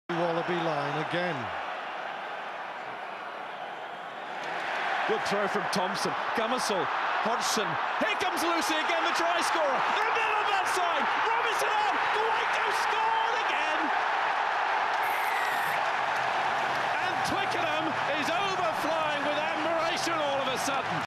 2004b - Australia AI Cueto.mpeg
Cueto's try against Australia in 2004